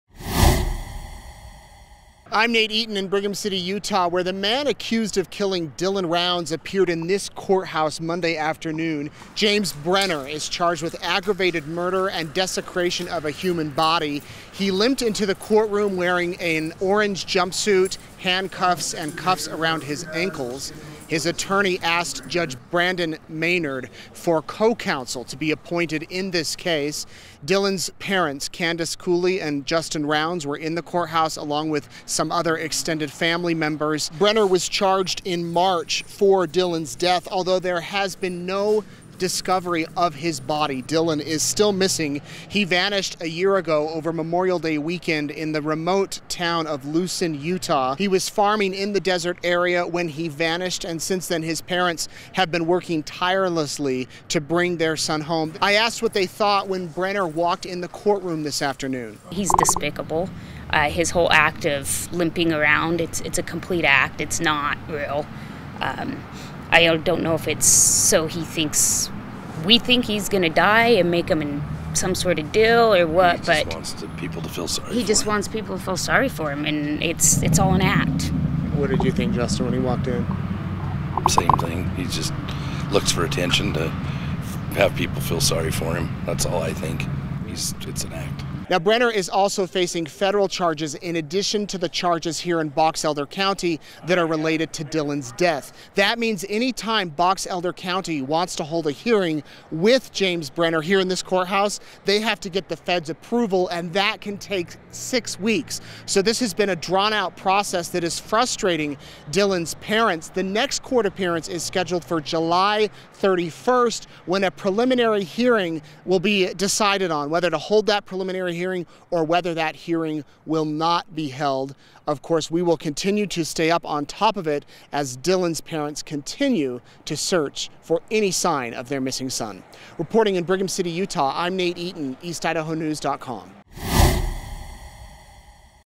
James Brenner, man accused of killing Dylan Rounds, appears in Utah court
BRIGHAM CITY, Utah -- The man accused of killing Dylan Rounds limped into a Utah courtroom Monday afternoon wearing an orange jumpsuit, handcuffs around his wrists and shackles on his ankles.

James Brenner spoke only one word during the nine-minute hearing in the Box Elder County Courthouse. First District Judge Brandon Maynard asked Brenner if he was okay with the timeline of future hearings and Brenner responded, "Yes."

The 59-year-old is charged with aggravated murder and abuse or desecration of a human body along with some firearms charges.

Brenner was squatting on property near Rounds’ trailer when the 19-year-old disappeared from his farm in the remote down of Lucin, Utah over Memorial Day weekend in 2022. His body has not been found but Rounds' boots were found near a dirt mound and his phone was discovered at the bottom of a pond near Brenner’s trailer.

Brenner has a violent criminal history and was arrested in June 2022 on outstanding warrants. He was booked into the Weber County Jail in Ogden, Utah on federal charges.

During Monday’s hearing, Michael Studebaker, Brenner’s attorney, asked Judge Maynard to appoint co-counsel to assist in the defense.

Maynard said he would look into the issue of having an additional defense attorney appointed and explained the challenges the court has to deal with as Brenner is facing both state and federal charges.

"Here's the problem that is presented in this case. Mr. Brenner is in federal custody and each time this court acts, it has to get permission from the federal government to bring Mr. Brenner in," Maynard said.

Box Elder County Attorney Blair Wardle said the process takes six weeks so Brenner's next court appearance, where a decision will be made on whether to hold a preliminary hearing, won't be held until July 31.

Candice Cooley and Justin Rounds, Rounds' parents, were in the courtroom with some extended family members and explained how they felt seeing Brenner walk into the hearing.

"He's despiacble. His whole act of limping around is a complete act. It's not real," Cooley told EastIdahoNews.com. "I don't know if it is so we think he's going to die or make him some sort of deal but he wants people to feel sorry for him."

Rounds added, "He looks for attention to have people feel sorry for him. That's all. I think it's an act."